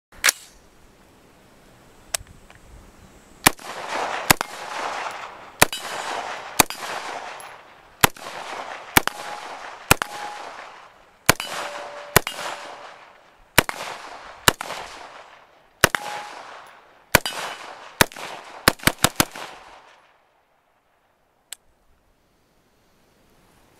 Mossberg MMR Tactical -- Field Testing
Nick Leghorn tests out the Mossberg MMR Tactical, the new AR-15 rifle from Mossberg.